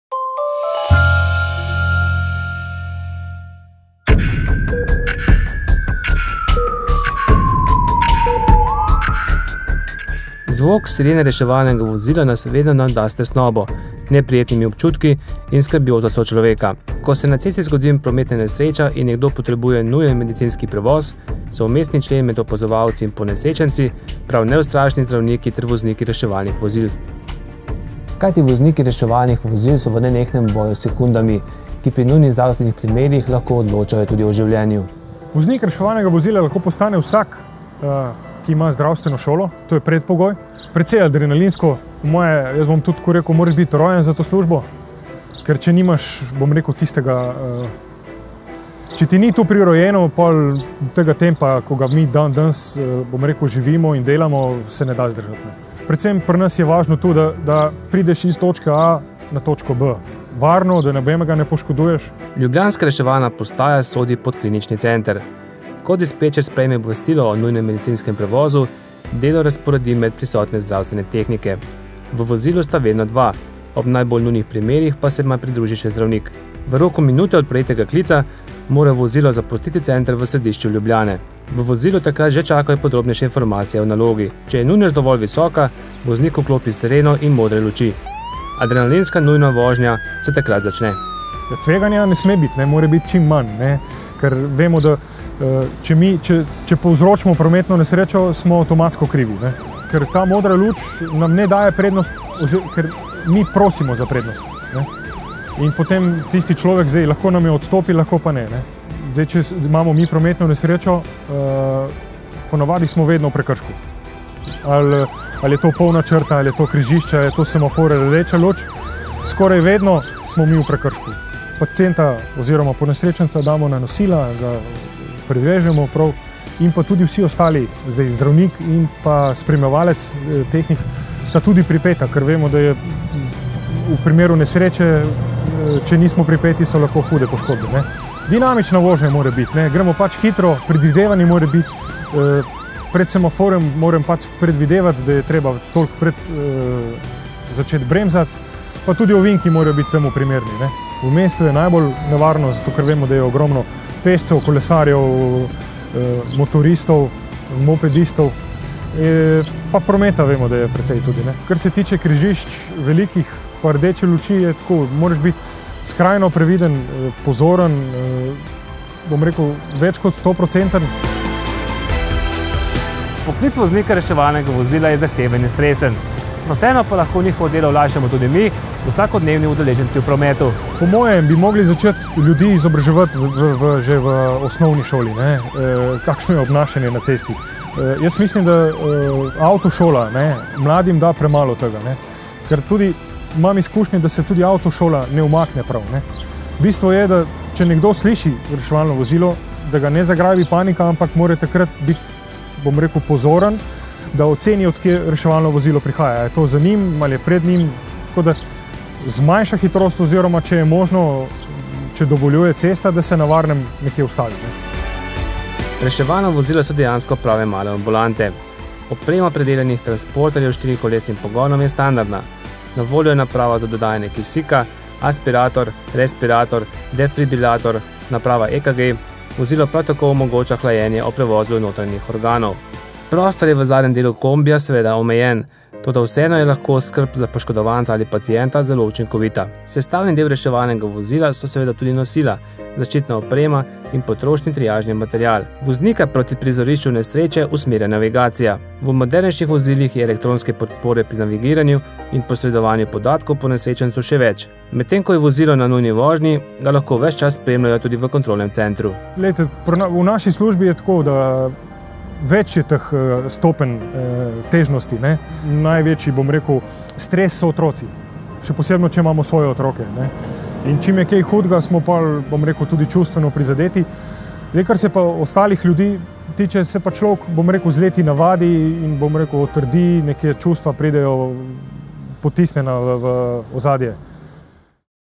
Reportaža: Vozniki reševalnih vozil
Voznik reševalnega vozila je za mnoge privlačna služba, toda vsakodnevni stres, odgovornost in soočenja s tragedijami zahtevajo svoj davek. Pogledali smo v zakulisje dela ljubljanskih reševalcev.

Avtor: Gregor Pavšič
Kamera in montaža: Dean Gostimirovič